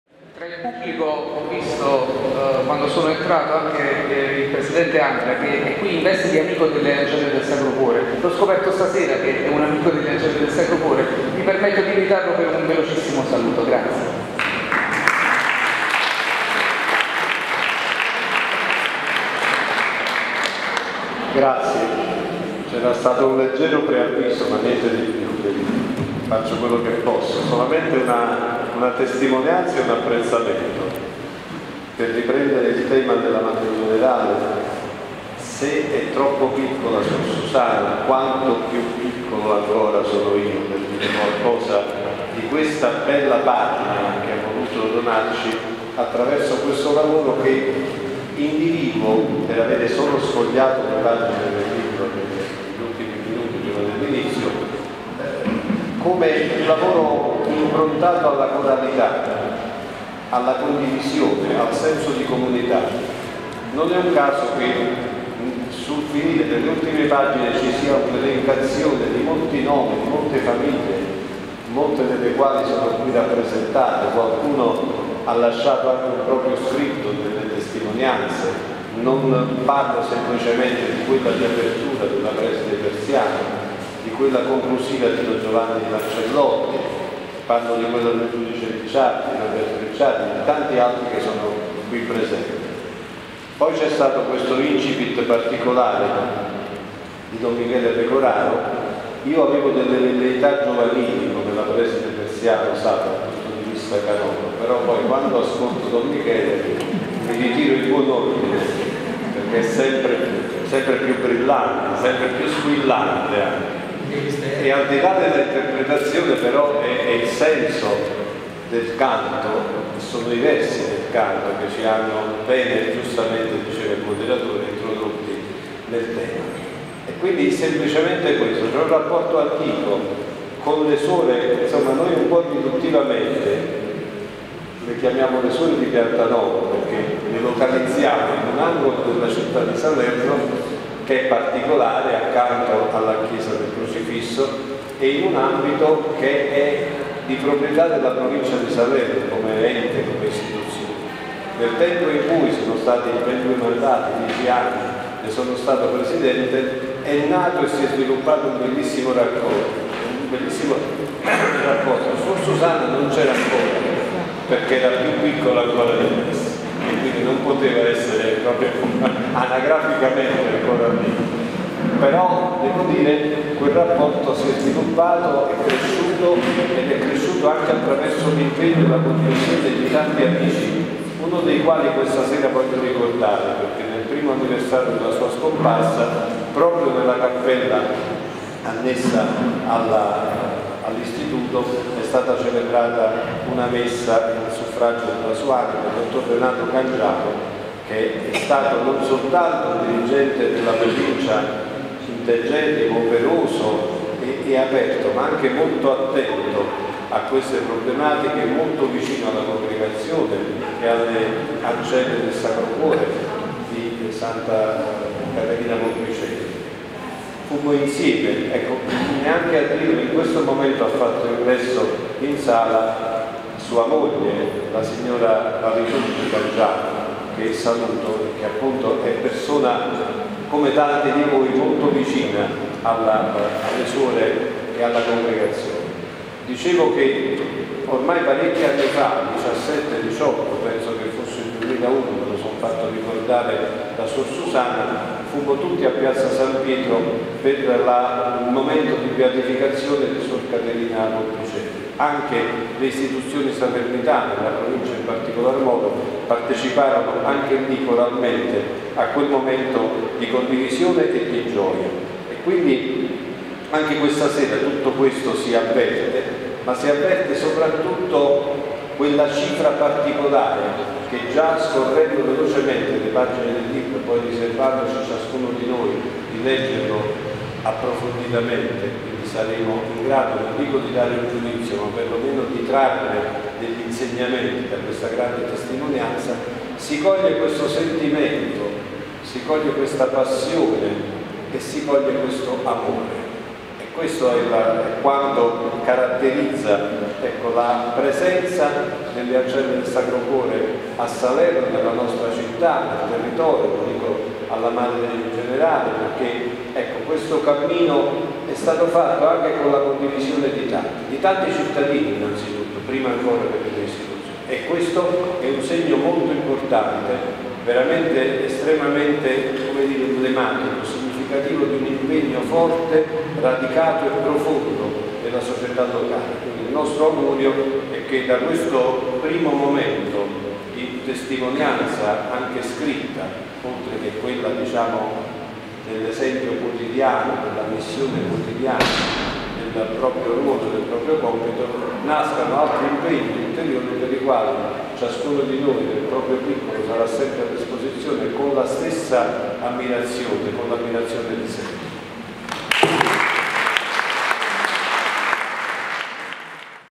Duomo di Salerno: "Presentaz.libro di Suor Susana" 25/6/19 n.4
Salerno, 25 Giungo 2019 ore 19.30
Duomo di Salerno - Sala San Tommaso
Presentazione del libro: "Troppo piccola per un amore così grande"
di Suor Susana Rodriguez Castillo
Intervengono:
Don Michele Pecoraro - Parroco del Duomo di Salerno
Dott. Dino Franco - Moderatore
Prof.ssa Emilia Persiano - Dirigente scolastico
Arch. Pina Russo

Suor Susana  - Autrice
L'intero incasso sarà dato in beneficenza:
"Santa Caterina" - Indonesia - Panama  e Brasile